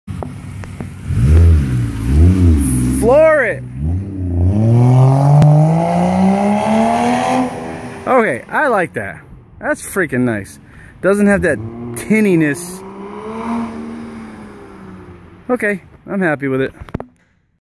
2007 Ford Edge SEL PLUS AWD
Removed three mufflers added one resonator